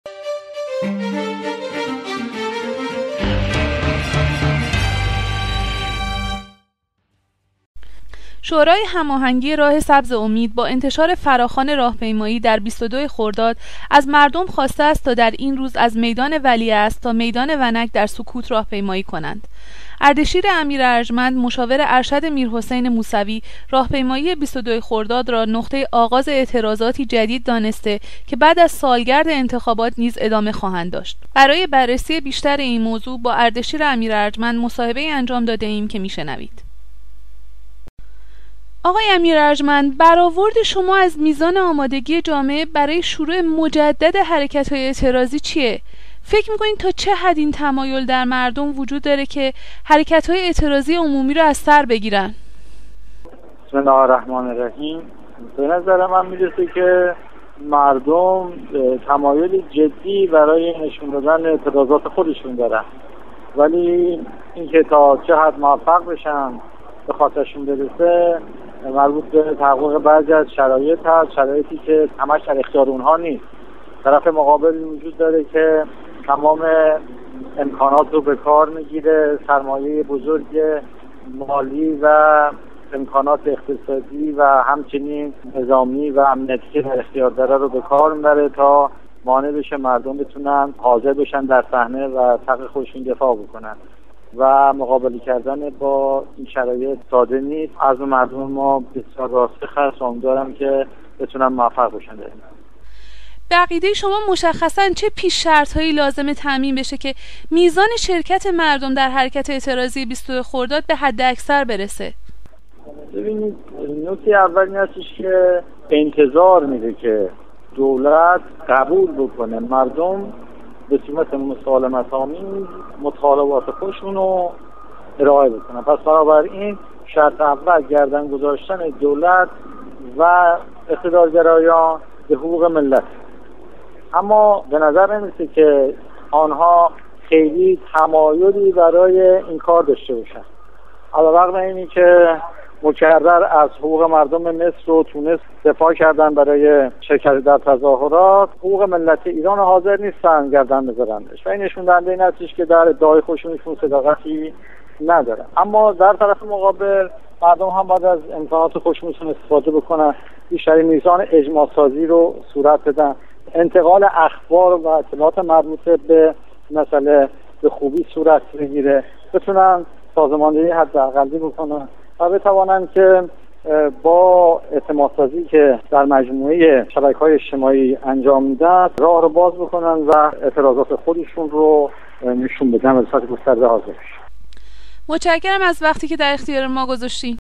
اردشیر امیرارجمند - فراخوان حرکت اعتراضی ۲۲ خرداد
مصاحبه با اردشیر امیرارجمند، مشاور ارشد میرحسین موسوی